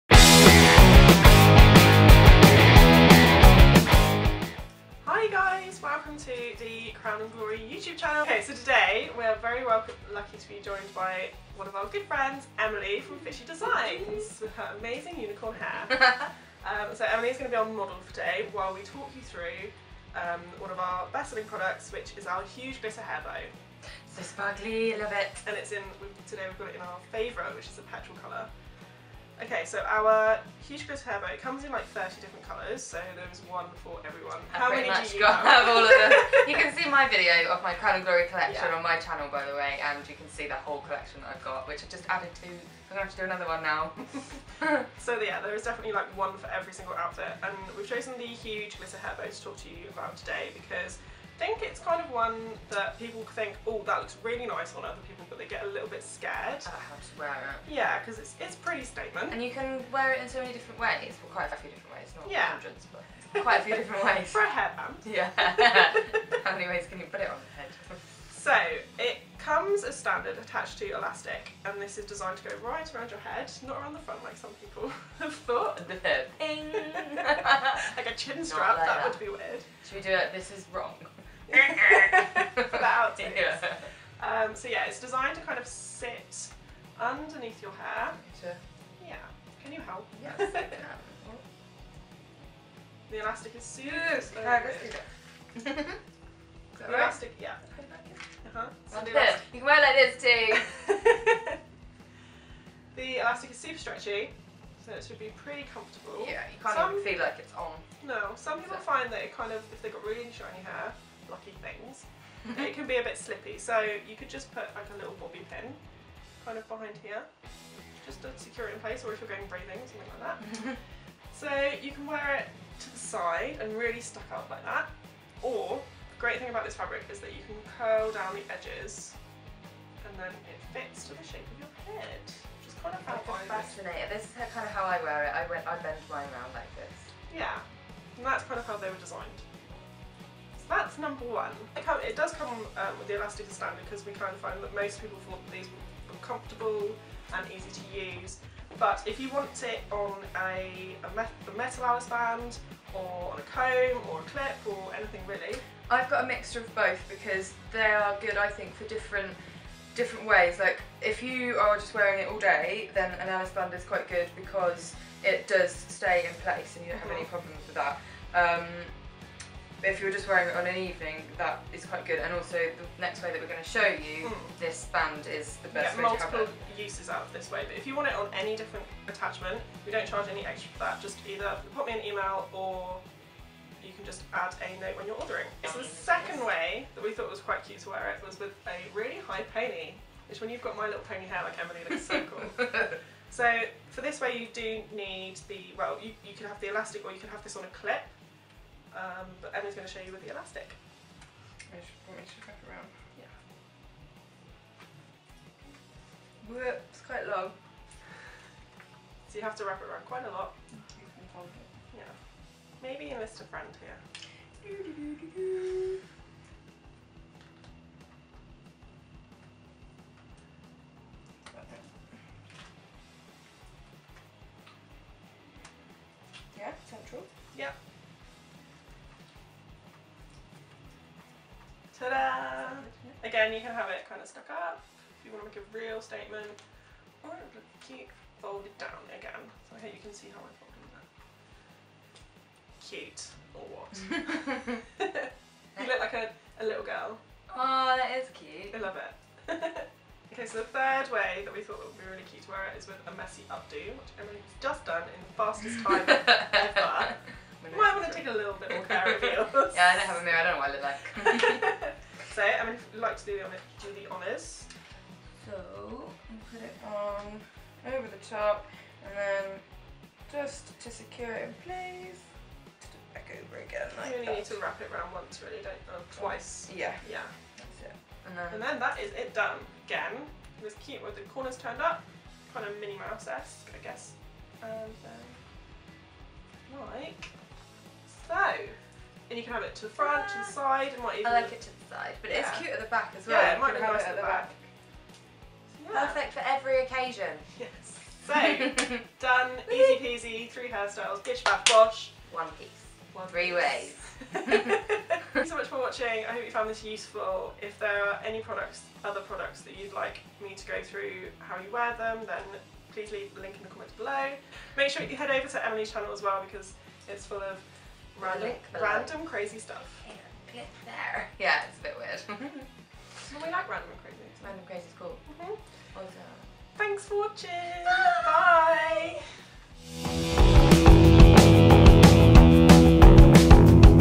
Crown and Glory Ways to Wear - Huge Glitter Bow
Crown and Glory Ways to Wear - Three ways to wear our Huge Glitter Bow